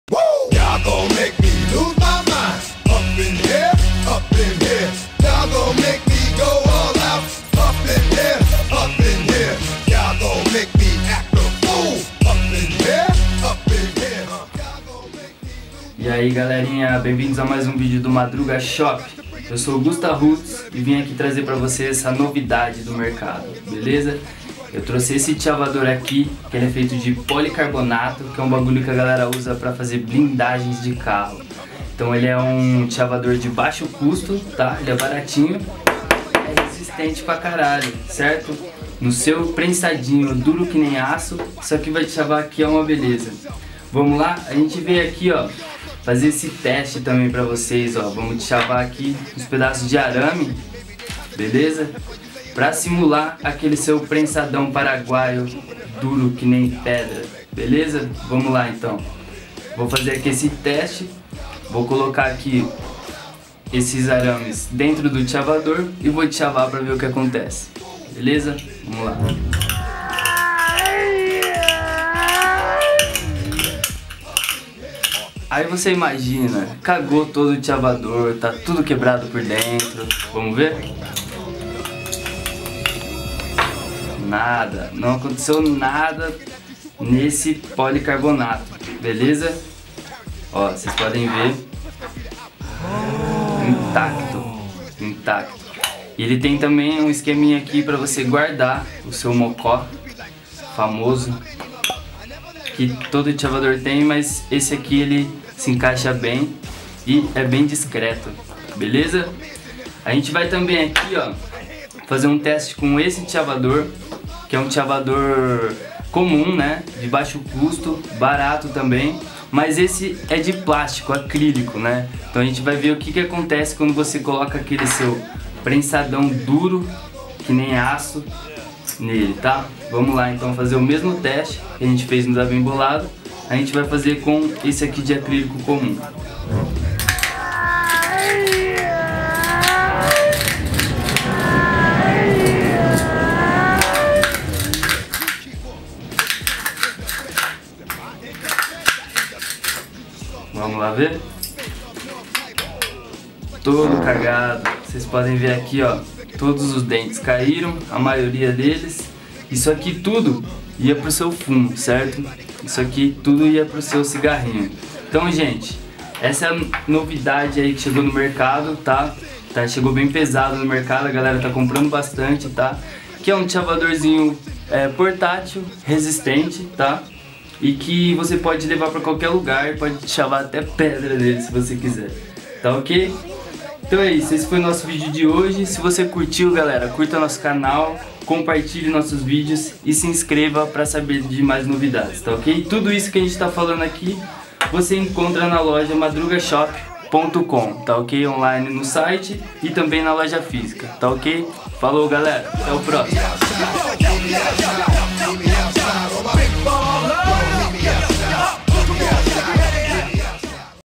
Dichavador de Policarbonato Bem Bolado - Dichava tudo!
Salve galerinha do Madrugashop, mais um vídeo no ar, desta vez testamos um Dichavador pesado da Bem Bolado feito de Policarbonato, perfeito pra dichavar aquele prensadão durão.

Madrugashop Tabacaria e Headshop mais completa do Brasil!